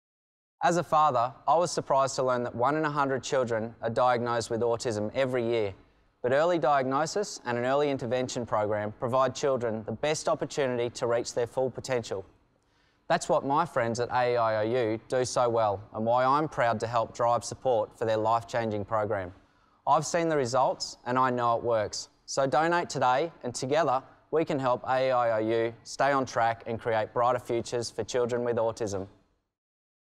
AEIOU Foundation and Lee Holdsworth
V8 Supercar driver Lee Holdsworth is an official ambassador of AEIOU Foundation - for children with autism. Listen to Lee tell his heartfelt message.

Based in Australia, AEIOU Foundation offers early intervention therapy and care for children with autism aged 2 - 6 years.